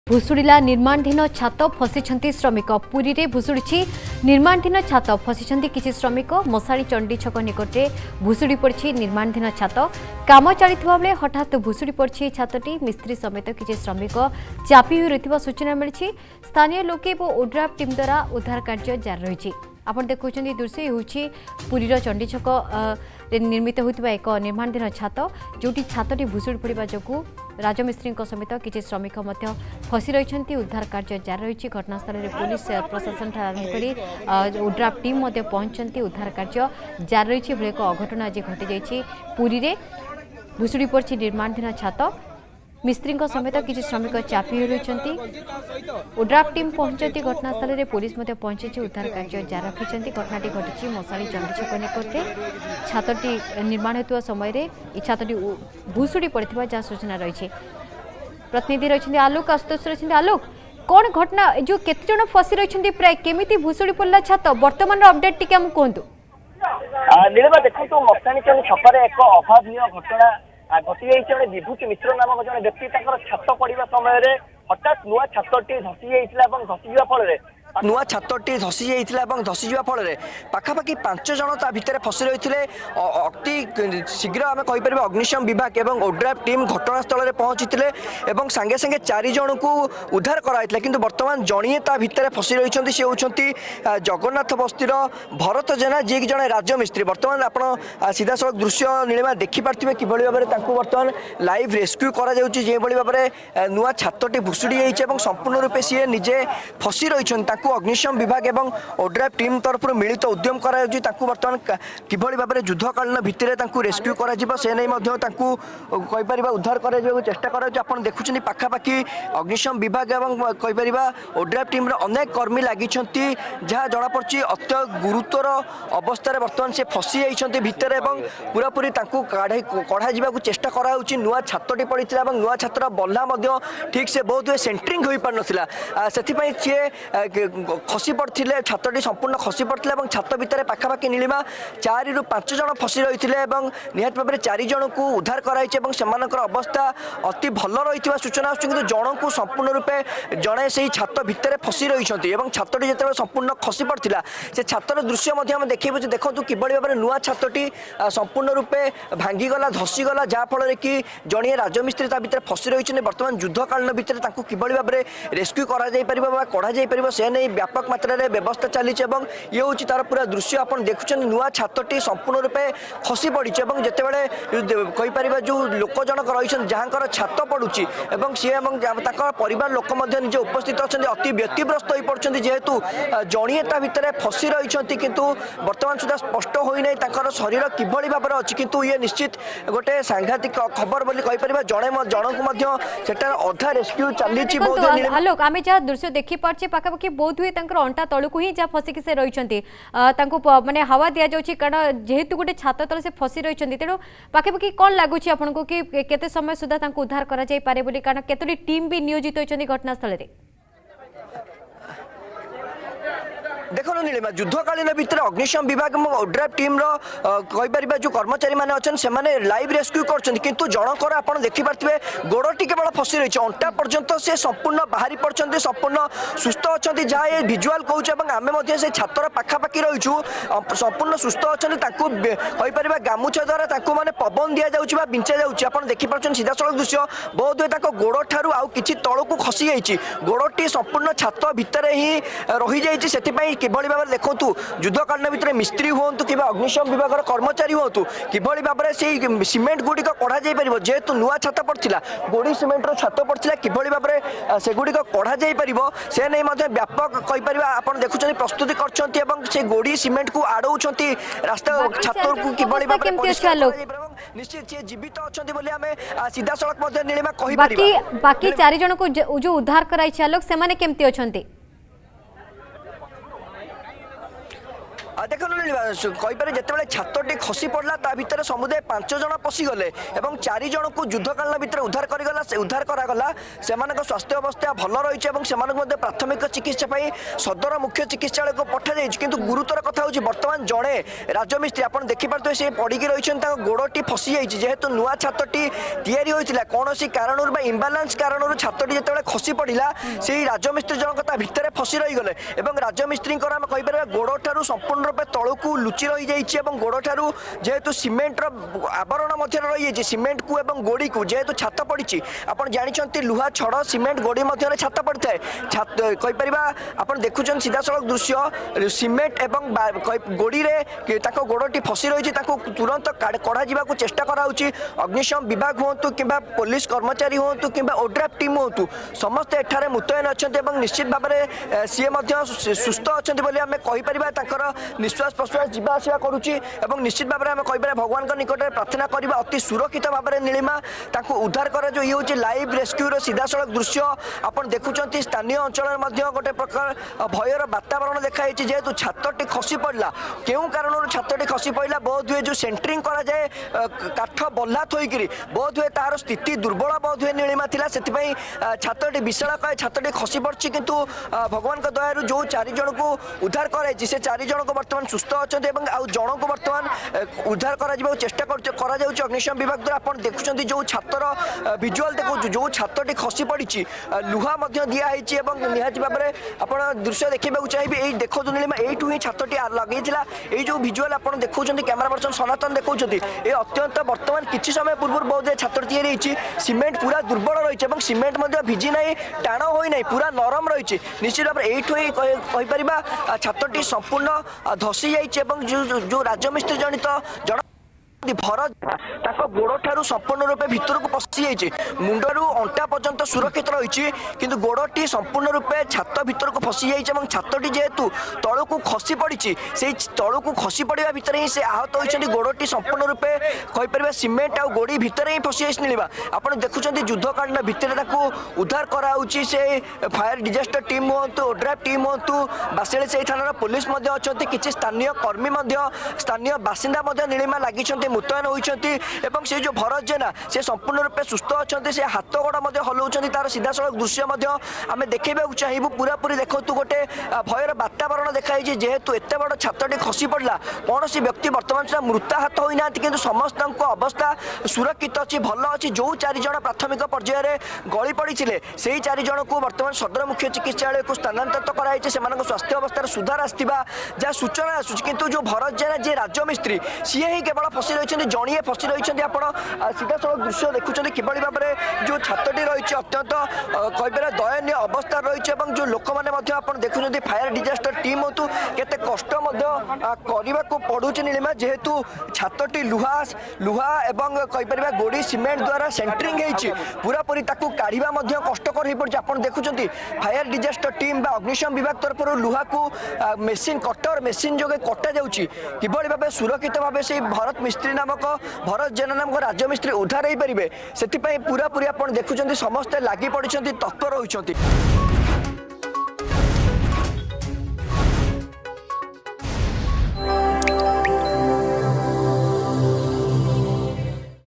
ଭୁଶୁଡ଼ିଲା ନିର୍ମାଣାଧୀନ ଛାତ, ଫସିଛନ୍ତି କିଛି ଶ୍ରମିକ
ମଶାଣି ଚଣ୍ଡୀ ଛକ ନିକଟରେ ଭୁଶୁଡ଼ିଲା ନିର୍ମାଣାଧୀନ ଛାତ
କାମ ଚାଲିଥିବାବେଳେ ହଠାତ୍ ଭୁଶୁଡ଼ି ପଡ଼ିଲା ଘର ଛାତ